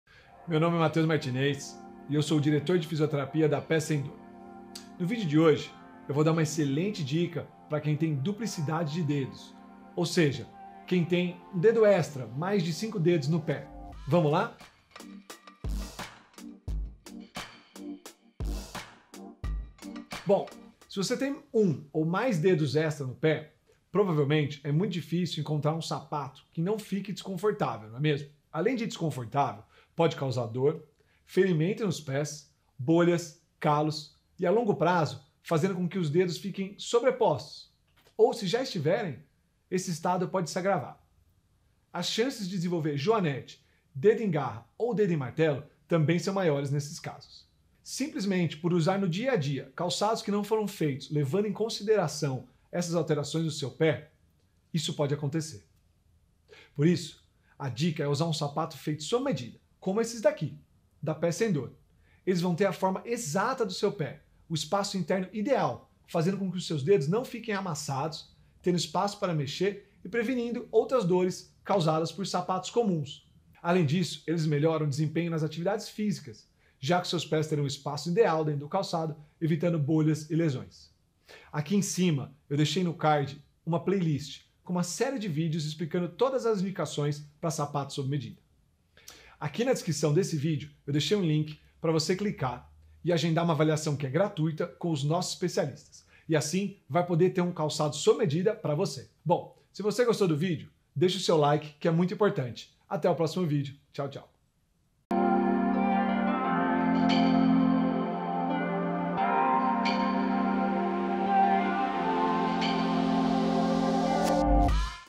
SAPATO SOB MEDIDA PARA DEDOS DUPLICADOS
Agende uma avaliação de seus pés, sem compromisso, no ou se preferir, envie um WhatsApp clicando nesse

Ter a duplicidade de dedos pode tornar a maioria dos sapatos desconfortáveis, mas não se preocupe, a Pés Sem Dor tem a solução para essa condição!

Acompanhe o vídeo até o final para entender como os sapato sob medida da Pés Sem Dor podem ser uma ótima solução.